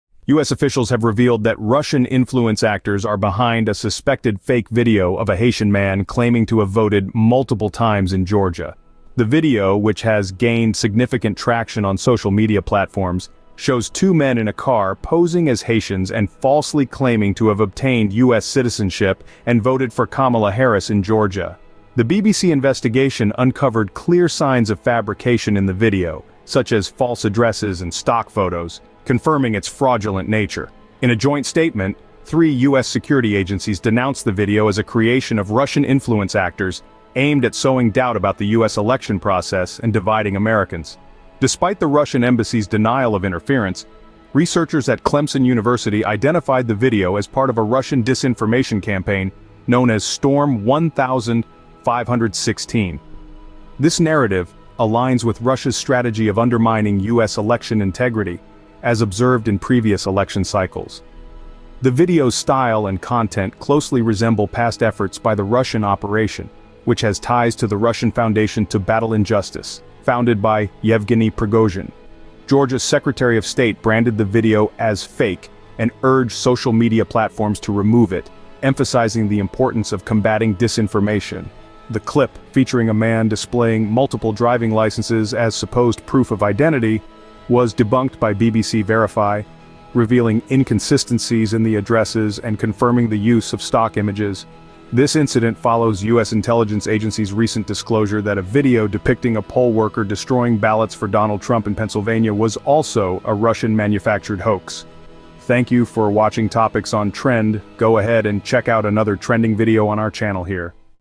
Unmasking the Fake The 'Haitian Voters' Video 2024
U.S. officials reveal a Russian-produced fake video attempting to mislead Haitian voters, marking a new twist in election interference tactics.